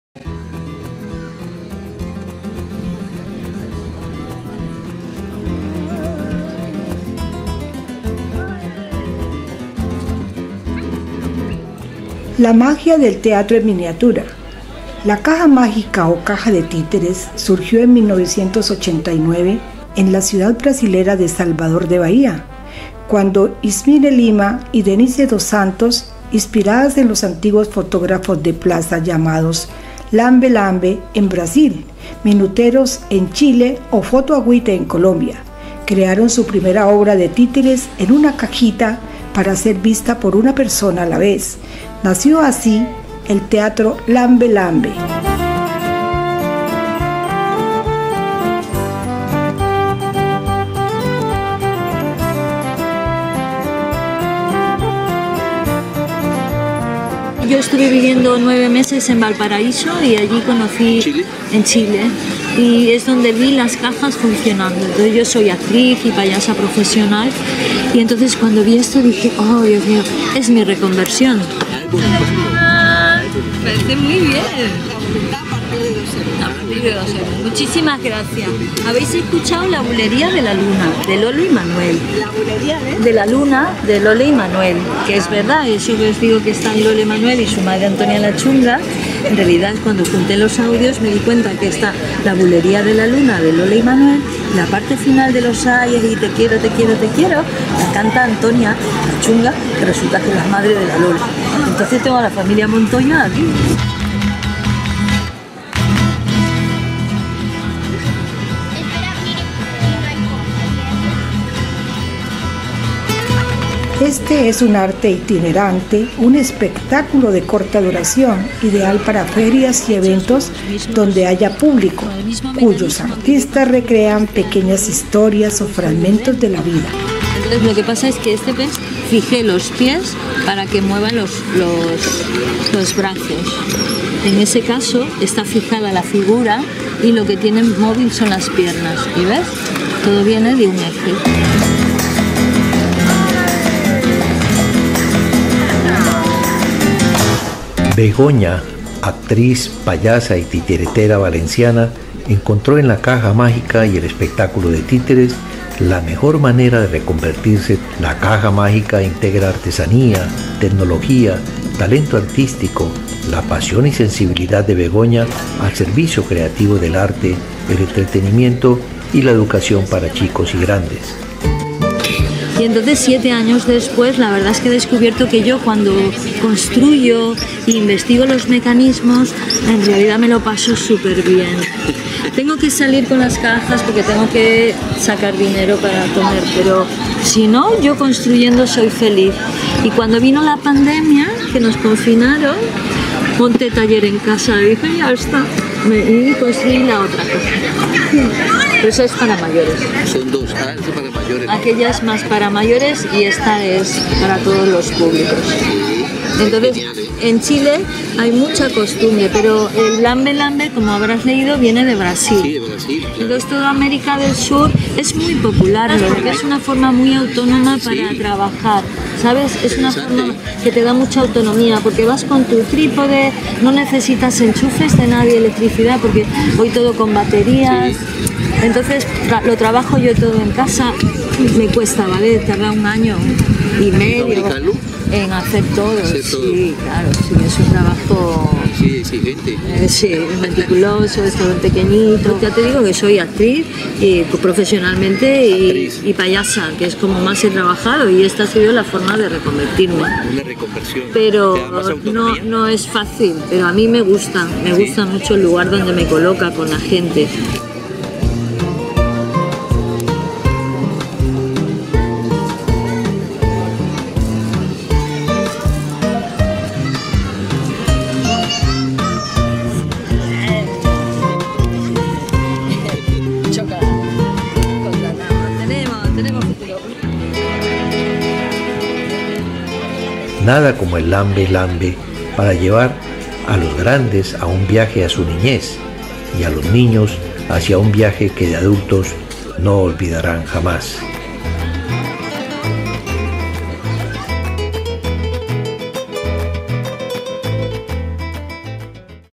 La Titiritera Feliz y reconvertida/Valencia, España
La titiritera feliz. Begoña es una titiritera, payasa y actriz española, quien tras durante un viaje a Chile conocio alli un artilugio que le cambiaria la vida para siempre. Ante ella se reveló una caja mágica que le significó reconvertir su vida  y hacer felices a chicos y grandes.